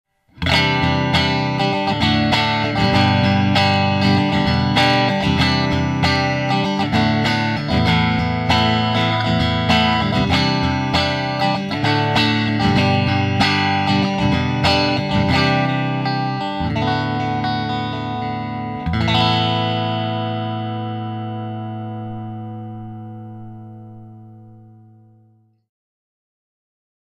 SPEAR Gladius-HT Blue Marble サウンドサンプル クリーン（ミックスータップ）
輝く水面のようなメタリックなマーブル感が美しいストラトタイプ。
SPEAR 2011 Winter モデル「グラディウスＨＴ」シリーズです☆
マホガニーボディにメイプルネック＆ローズ指板。
24F＆セットネック仕様でハイポジまでスムーズなプレイアビリティ。

ピックアップはEMGーHZハムバッカーをフロント、リア共に搭載。
しかも、コイルタップによりシングル化も可能！
分厚いサウンドから繊細なサウンドまでいけちゃいます♪

サウンドはすべてアン直です！
使用アンプ：Fender Hotrod DeluxeⅠ

なんだか普通じゃない楽器屋 ギターマニア♪